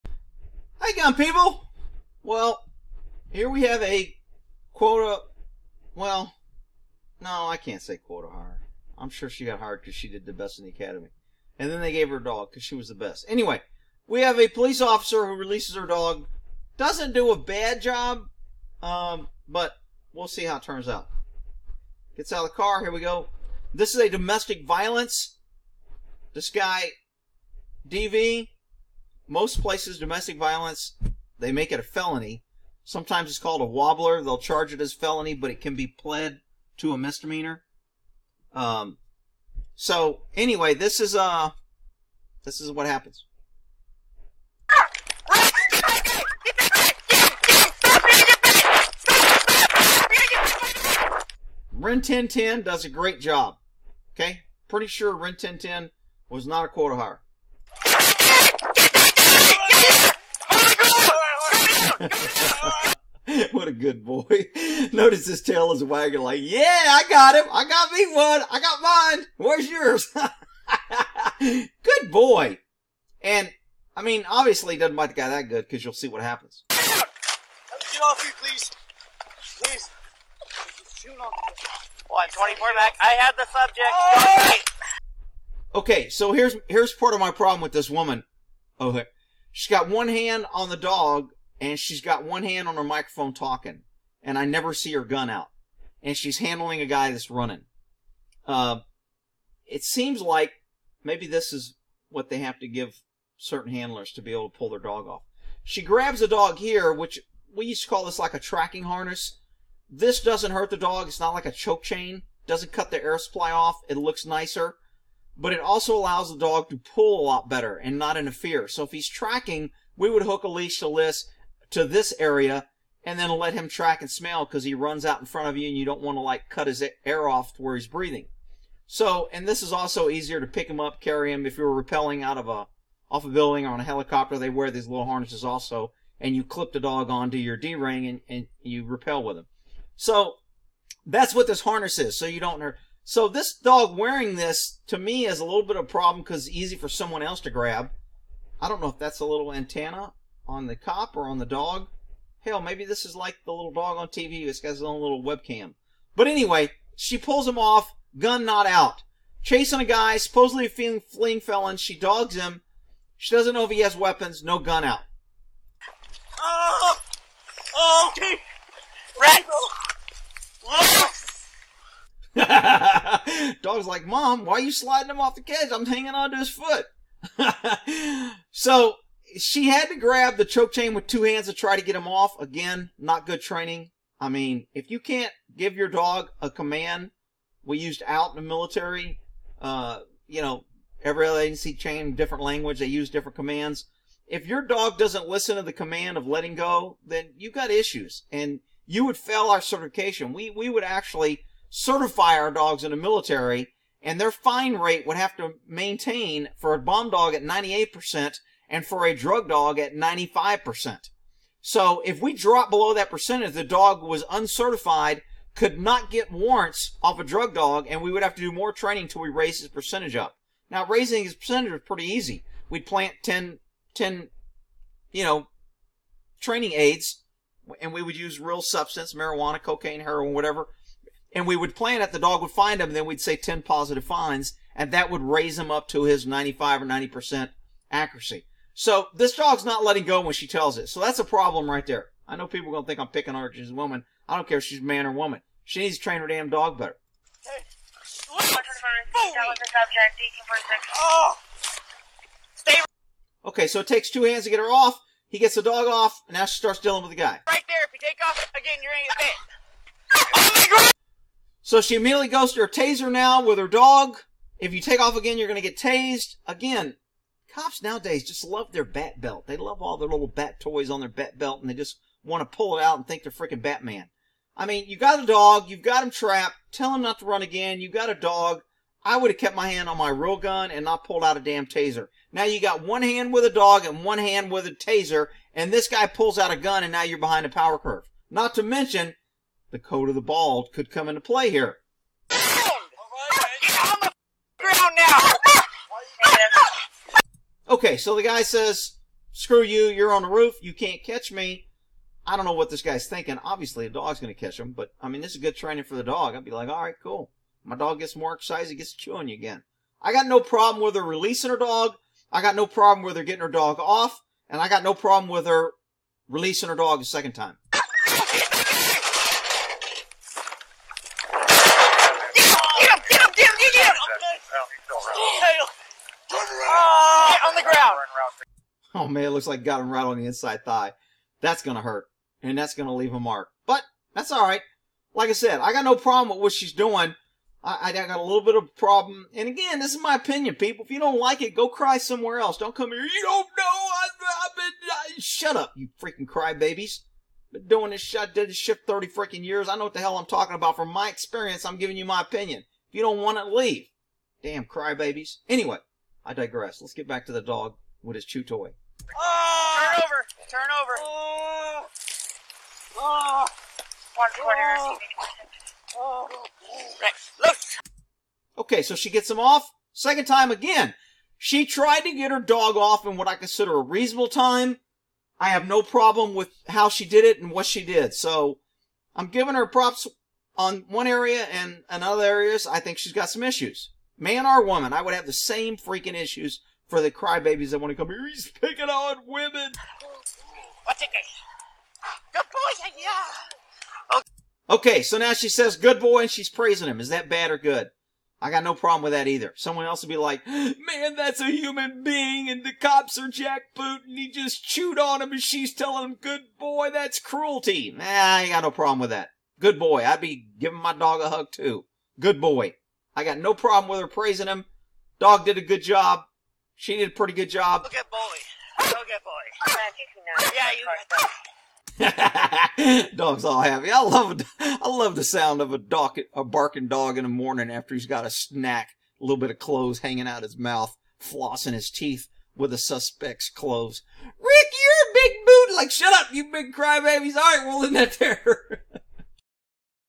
Female Cop Releases Police K-9 Twice On Same Suspect - Some Good & Bad
For the most part I am OK with this use of police dog. NOTE: MY TWITTER IS  @Horsyguy